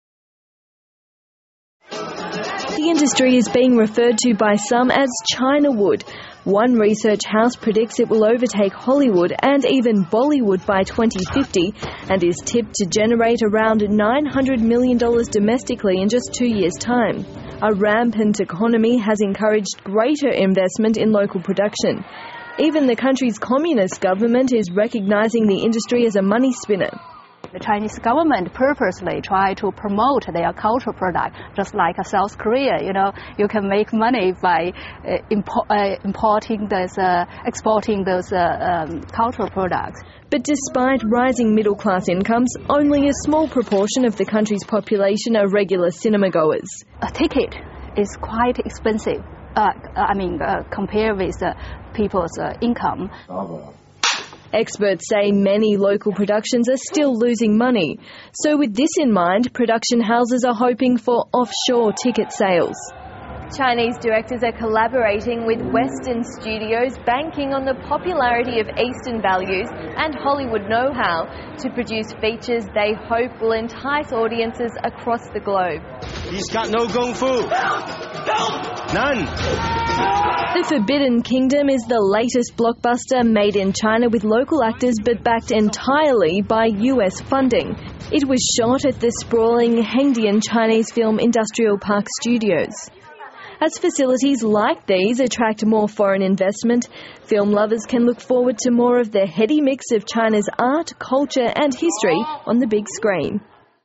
'China-wood' predicted to take centre stage
China's massive economic boom has ignited an entertainment industry which some analysts say could over-take Hollywood by 2050.